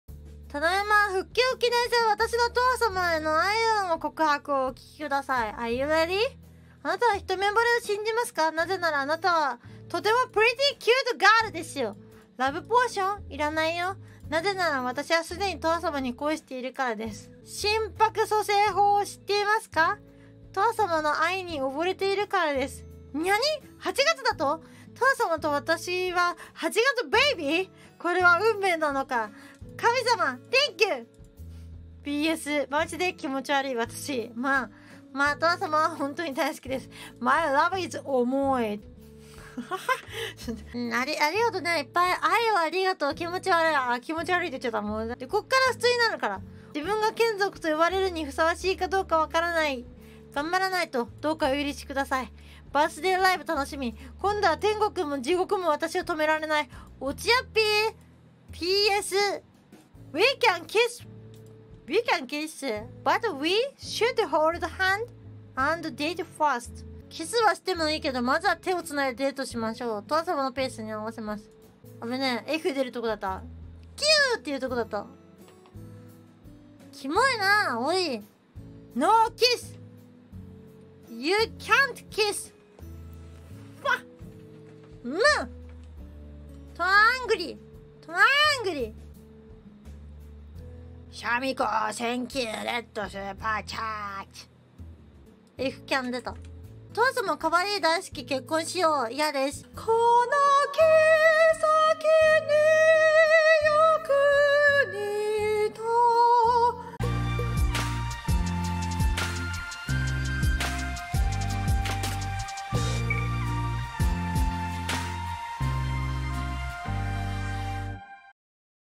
Towa Got Mad At Disgusting Red Superchats【Hololive | Eng Sub】
Towa sama became angry after getting disgusting red super chats from kenzoku.

「Tokoyami Towa」
Towa Ch. 常闇トワ

[Thumbnail Art]: -